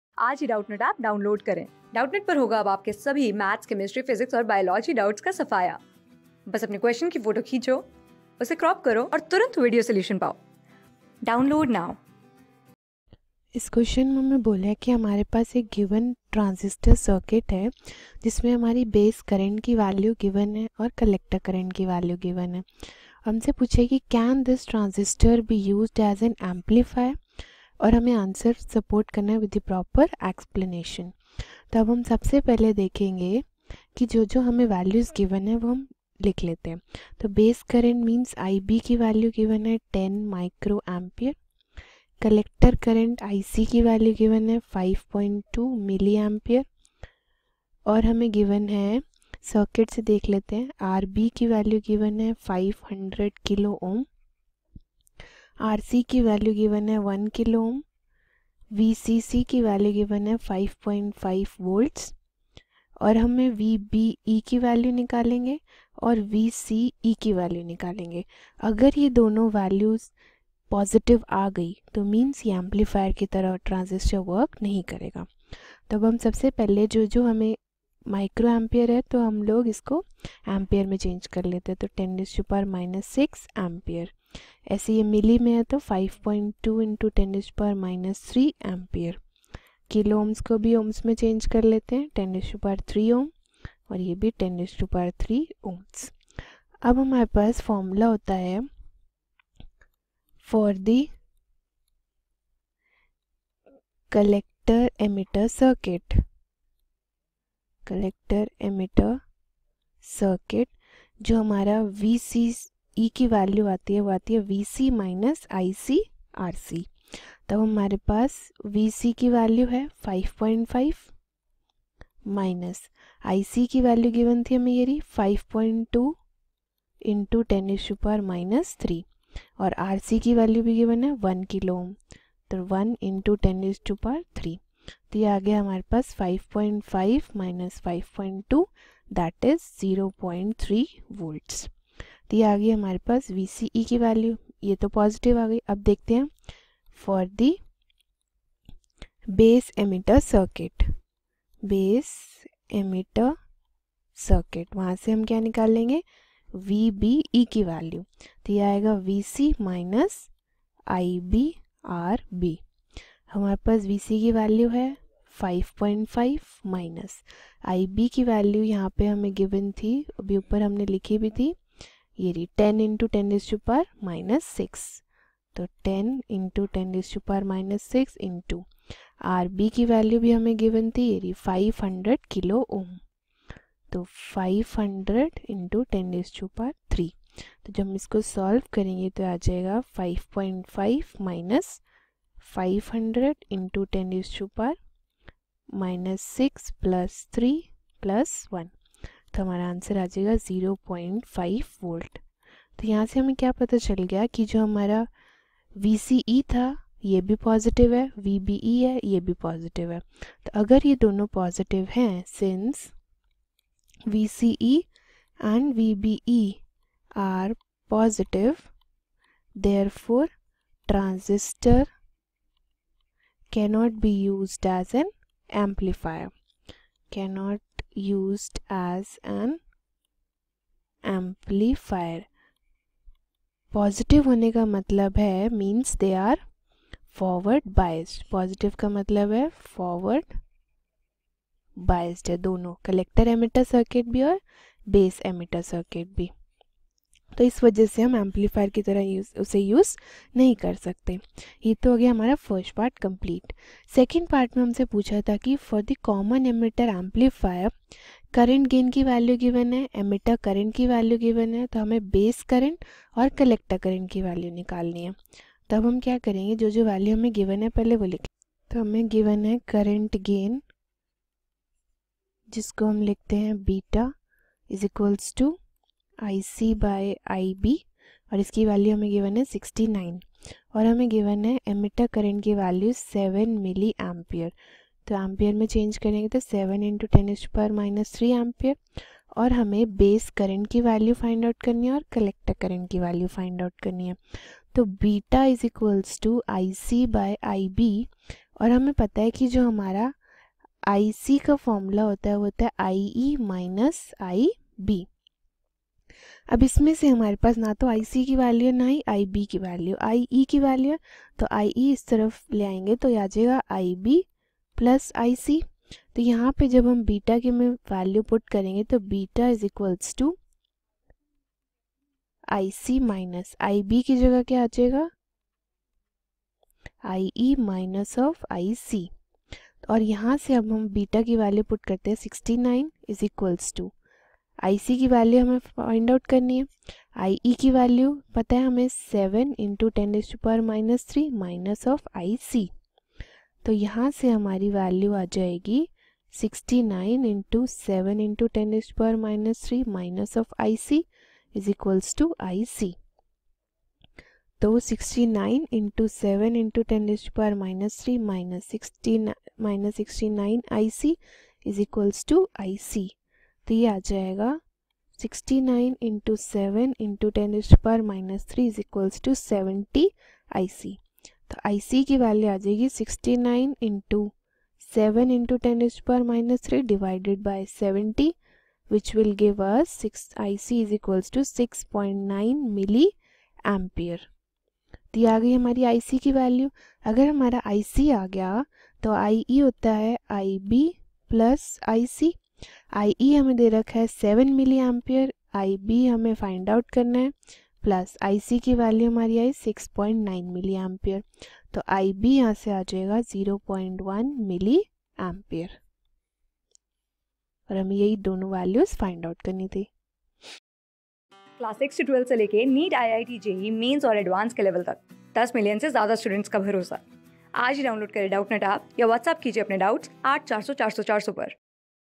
a) For given transistor circuit, the base current is `10muA` and the collector current is 5.2 mA.
(a) For given transistor circuit, the base current is `10muA` and the collector current is 5.2 mA. Can this transistor circuit be used as an amplifier. Your answer must be supported with proper explanation. (b). For a common emitter amplifier, current gain is 69. if the emitter current is 7 mA then calculate the base current and collector current.